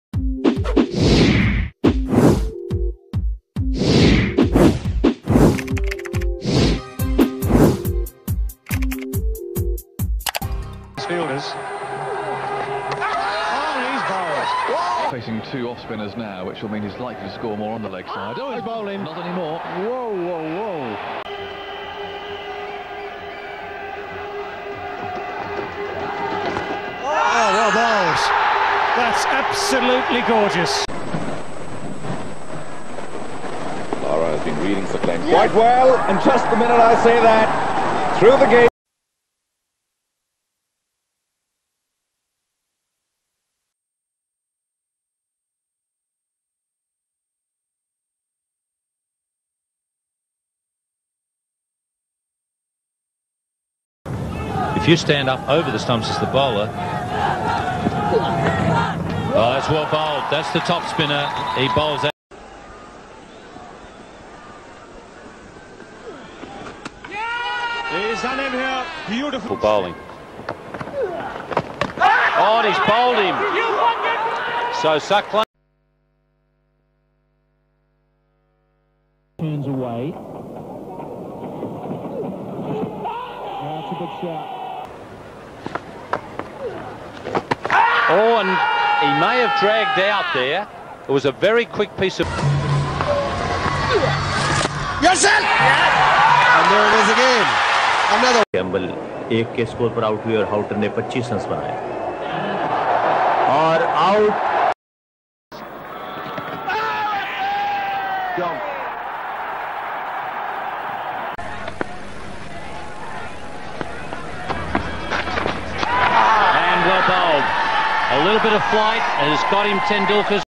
How to Bowl Doosra - Explained by Saqlain Mushtaq
Saqlain mushtaq invented doosra,people called it top spinner but then it was named doosra, very useful delivery for modern day off spinners to learn and get better opportunities